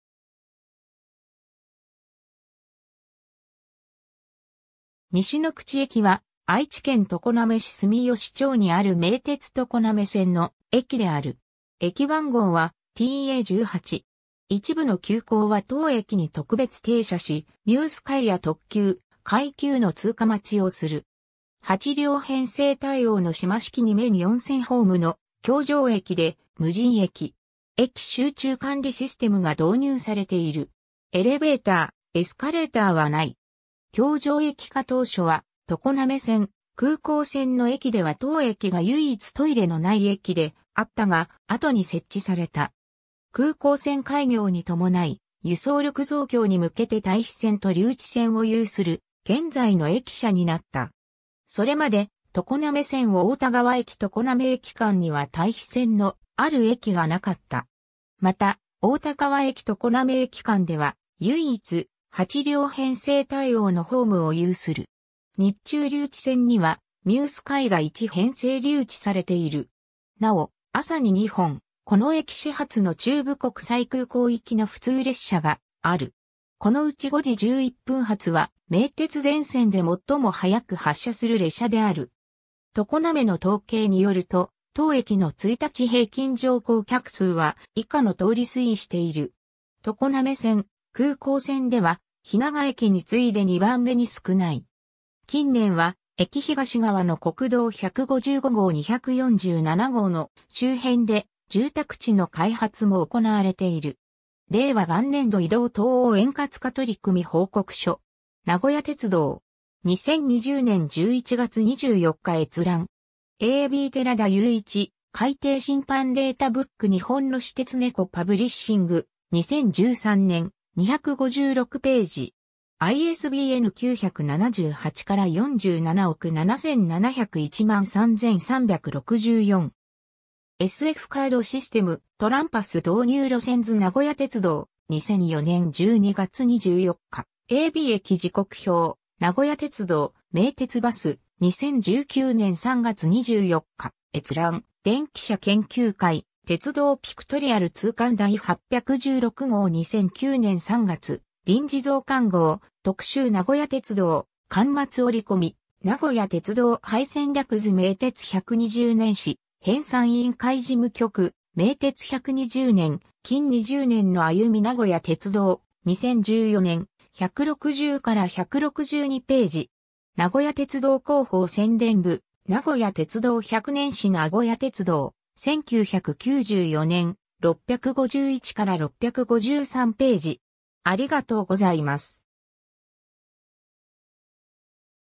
西ノ口駅
西ノ口駅（にしのくちえき）は、愛知県常滑市住吉町にある名鉄常滑線の駅である。
駅番号はTA18。
一部の急行は当駅に特別停車し、ミュースカイや特急、快急の通過待ちをする。
 8両編成対応の島式2面4線ホームの橋上駅で無人駅。
駅集中管理システム（管理駅は常滑駅）が導入されている。
エレベーター、エスカレーターはない。
橋上駅化当初は常滑線・空港線の駅では当駅が唯一トイレのない駅であったが、後に設置された。
 空港線開業に伴い、輸送力増強に向けて待避線と留置線を有する現在の駅舎になった。
それまで常滑線太田川駅 - 常滑駅間には待避線のある駅がなかった。
また太田川駅 - 常滑駅間では唯一、8両編成対応のホームを有する。
日中留置線にはミュースカイが1編成留置されている。
 なお、朝に2本（5時11分と5時35分）、この駅始発の中部国際空港行きの普通列車がある。
このうち5時11分発は名鉄全線で最も早く発車する列車である（常滑駅から来た回送が当駅から折り返して営業運行に入る）。
 「とこなめの統計」によると、当駅の一日平均乗降客数は以下の通り推移している。
 常滑線・空港線では、日長駅に次いで2番目に少ない。
 近年は駅東側の国道155号・247号の周辺で住宅地の開発も行われている。
 ^ “令和元年度 移動等円滑化取組報告書(鉄道駅) (PDF)”. 名古屋鉄道. 2020年11月24日閲覧。
 ^ a b 寺田裕一『改訂新版 データブック日本の私鉄』ネコ・パブリッシング、2013年、256頁。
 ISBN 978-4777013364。
 ^ SF カードシステム「トランパス」導入路線図 - 名古屋鉄道、2004年12月24日 ^ a b 駅時刻表：名古屋鉄道・名鉄バス、2019年3月24日閲覧 ^ 電気車研究会、『鉄道ピクトリアル』通巻第816号 2009年3月 臨時増刊号 「特集 - 名古屋鉄道」、巻末折込「名古屋鉄道 配線略図」 ^ 名鉄120年史編纂委員会事務局（編）『名鉄120年:近20年のあゆみ』名古屋鉄道、2014年、160-162頁。